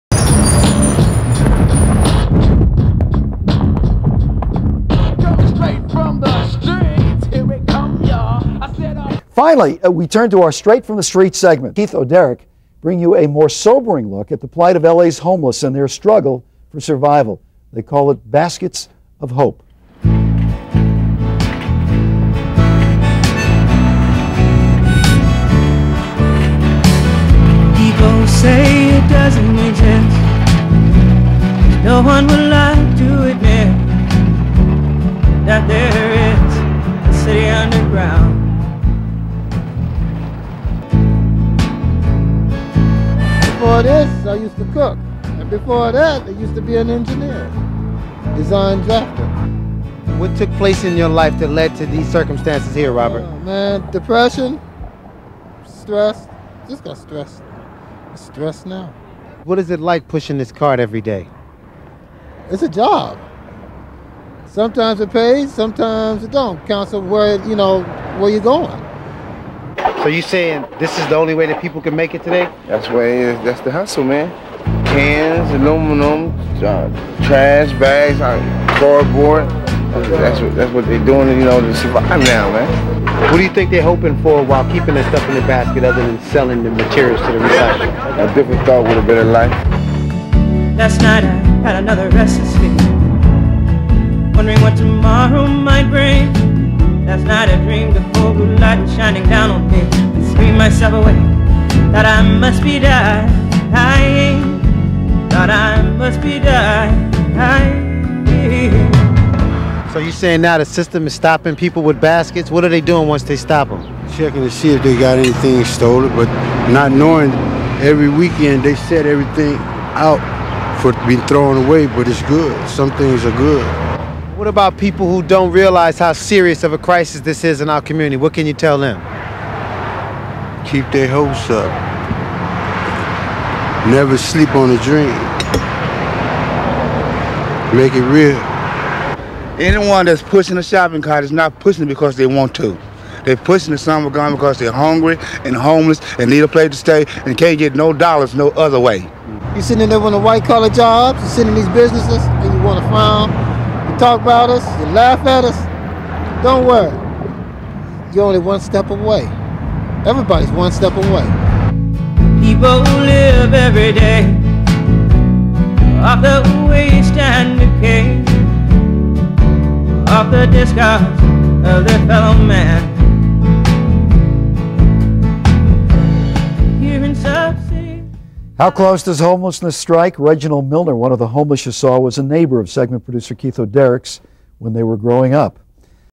Homeless with a Voice - Baskets of Hope - Straight from the streets
Homelessness in Los Angeles, CA has not changed. People talk  about the men and forget about the women and their children, especially the kids.
Being overlooked by the political system is not the solution.
Listen to these individuals and you will soon realize they are just people trying to survive on the mean streets. Robert Bartley was the next door neighbor of Straight from the streets producer Keith O'Derek and he spotted Robert pulling his shopping cart in downtown LA.
With camera in hand, Keith asked Robert to share his amazing story with the World.
Watch the video and just his remember Robert's poignant message at the end. This story was produced and shot by filmmaker award winning filmmaker Keith O'Derek in 1995. This is another reason why, Straight from the streets should still be on television today!
Baskets of hope was first coined by Keith O'Derek.